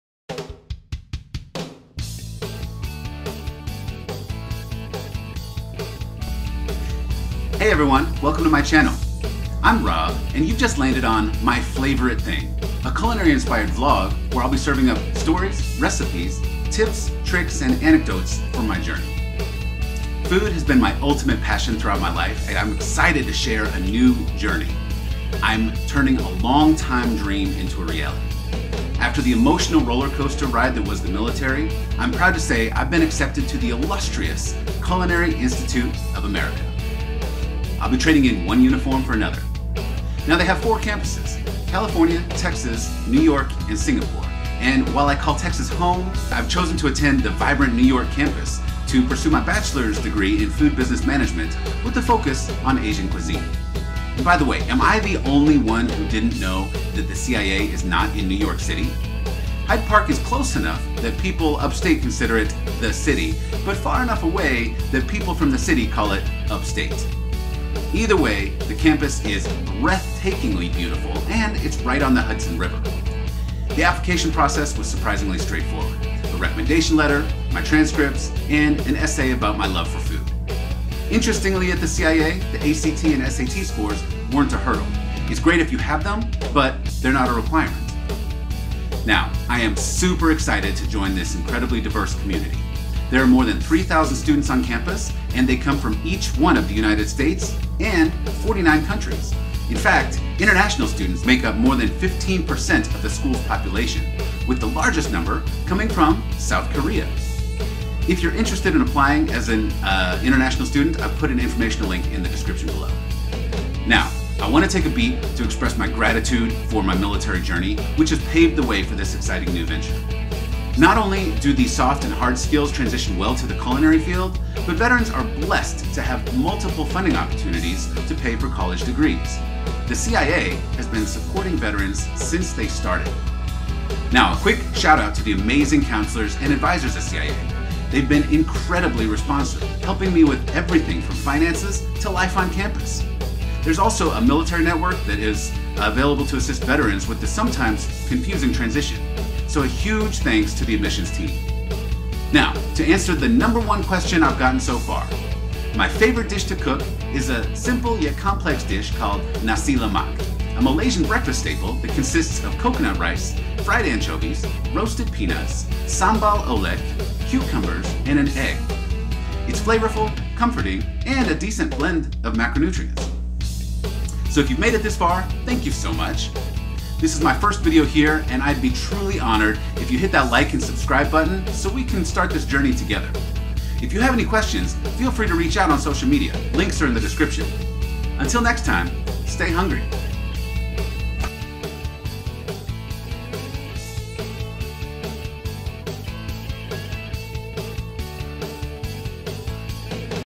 From Camo To Culinary w/ MyFlavoriteThing
From Camo to Culinary: My Journey from the Military to the Culinary Institute of America

Join me as I share the transition from military service to pursuing my passion for culinary arts at the renowned Culinary Institute of America (CIA).  In this video, I delve into how I traded my uniform for an apron, transitioning from the rigors of military life to the creative and flavorful world of the culinary arts.

Explore the application process that led me to the CIA's prestigious program, and learn how I'm embracing this exciting chapter of my life with open arms and a zest for learning. If you're someone considering a similar transition or simply intrigued by the journey, this video offers insights and inspiration.

Join me on this savory expedition as I blend my military past with a flavorful future. Your support means the world to me!